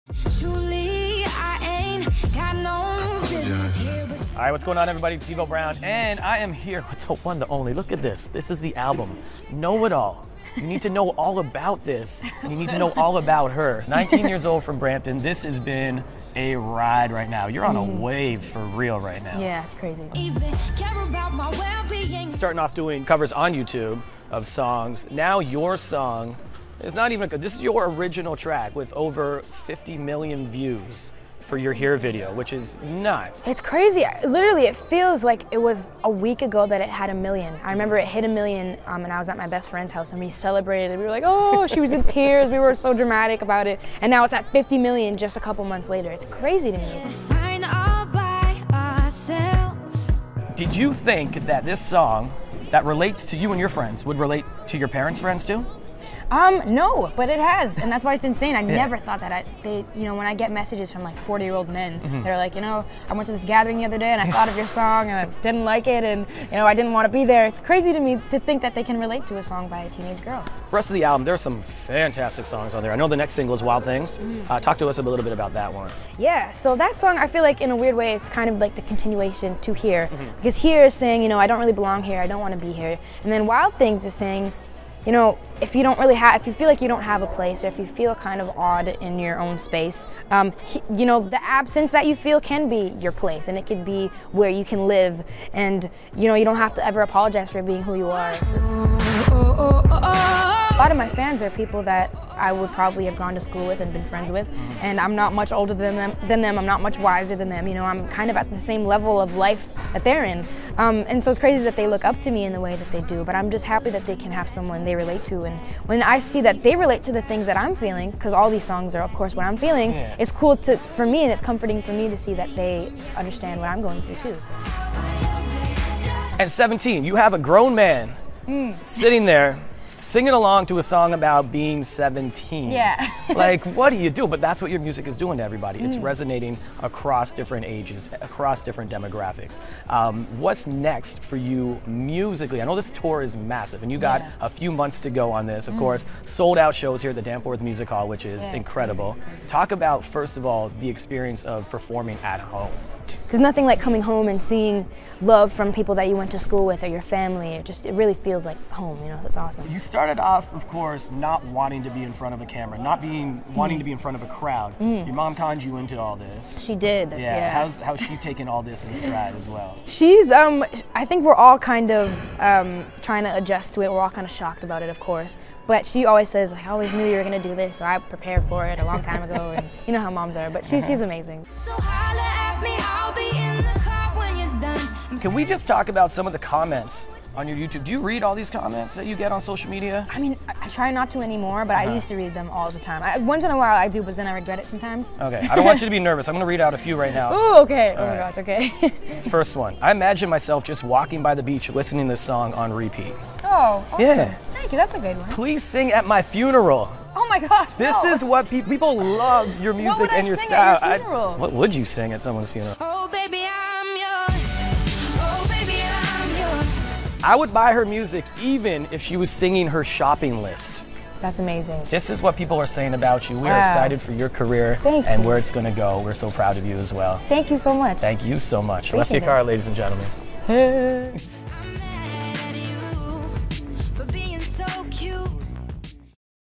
Video: Devo Brown’s KiSS 92.5 Weekly Top 3 songs: Jan. 17, 2016
Devo Brown takes a look at the top three songs from the past week as well as new music for the upcoming week.